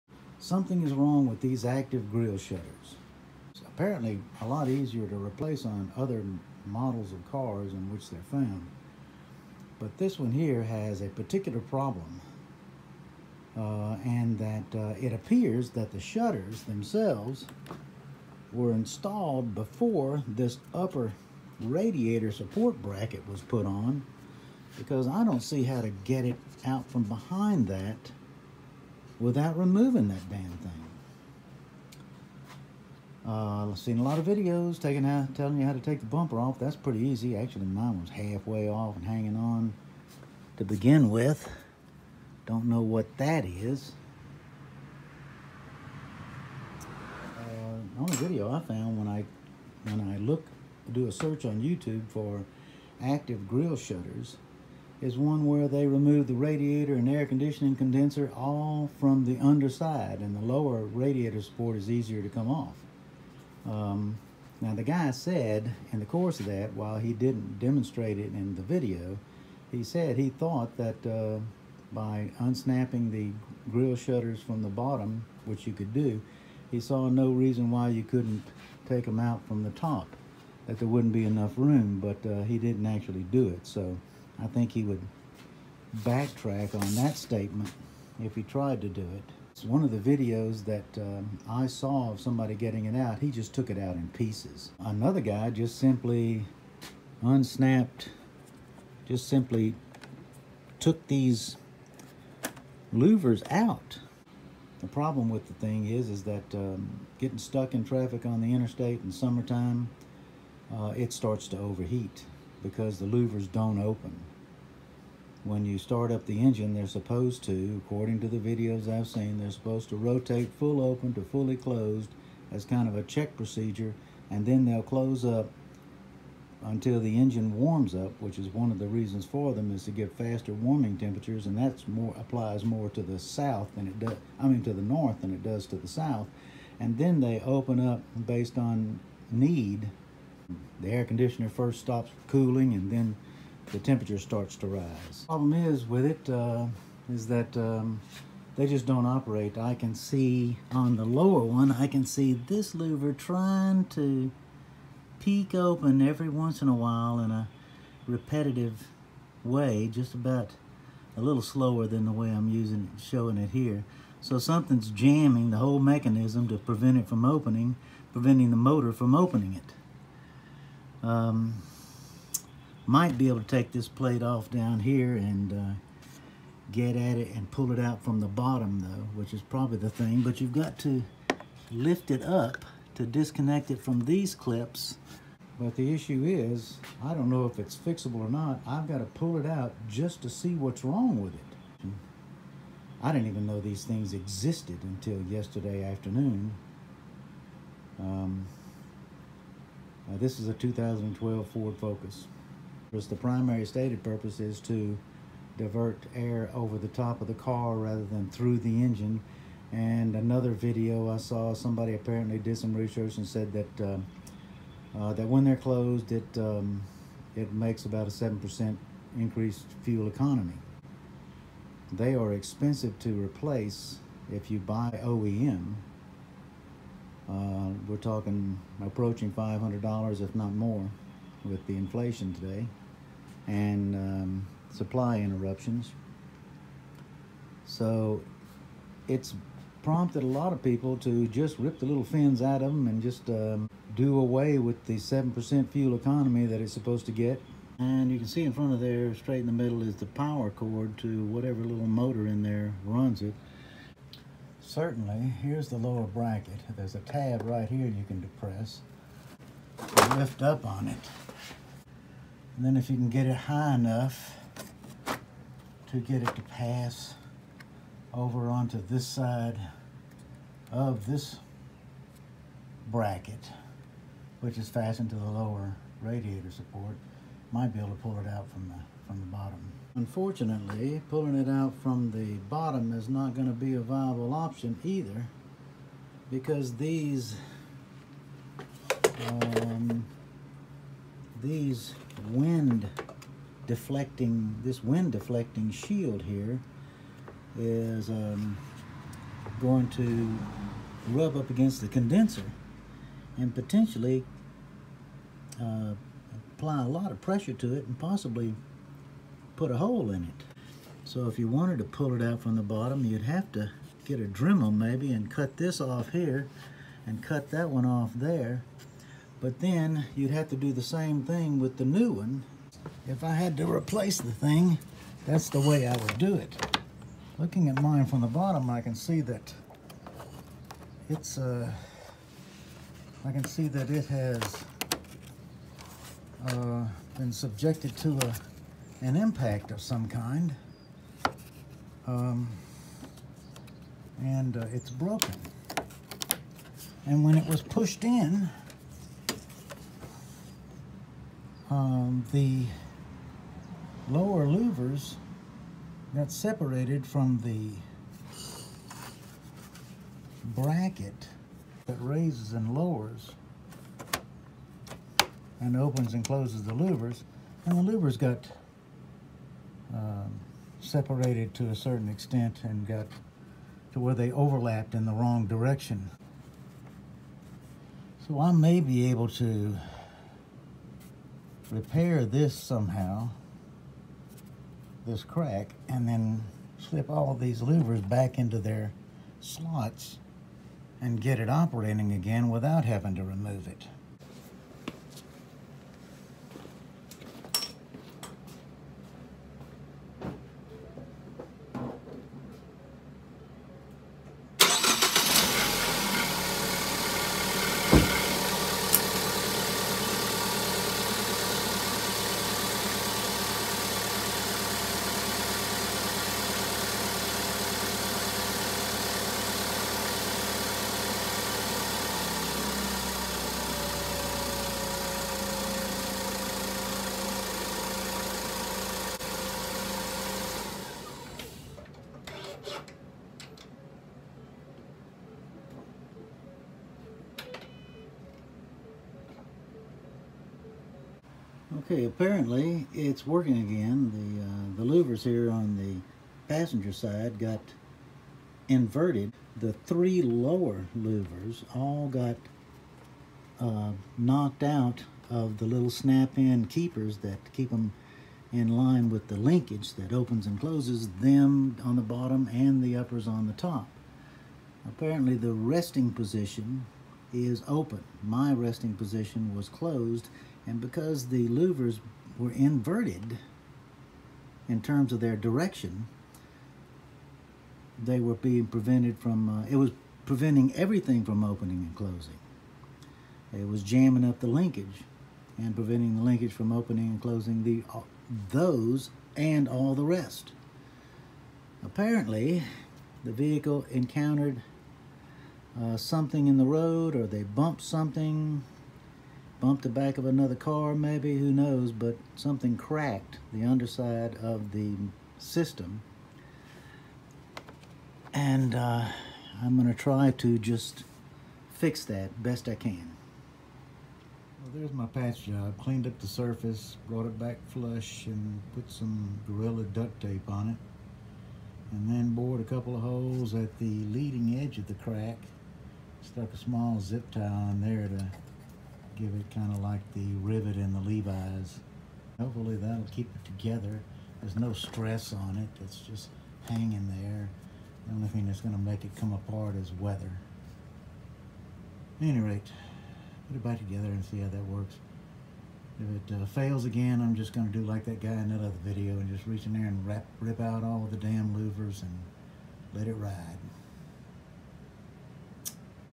How I Fixed My 2012 Focus Active Grill Shutters
Car overheated while stopped in heavy traffic on the interstate. Outside temps were hot but not real hot. This is not something that manifested itself at any other time driving. Turns out the active grill shutters were stuck close blocking air moving through the condenser and radiator. First the AC stopped cooling, then the engine started overheating. Normal operation is when the car is started the shutters operate to test function then close to warm the engine faster. Afterwards they may open and close with the temperature depending on the season as does the electric cooling fan. I imagine if the fan comes on the shutters open or what’s the point - would have to mount a camera in the grill to see how they operate while driving. If you turn on the AC, by default, the fan comes on and the shutters open. I assume that is the state of things as long as the AC is on. In the Deep South we run with the AC on almost all the time - even the defroster and heater, in climate control systems, uses the compressor to dry the hot air, so the shutters are open almost all the time - an assumption on my part. That means we will rarely get marketed fuel economy and environmental benefits of the system only attainable when the shutters are closed.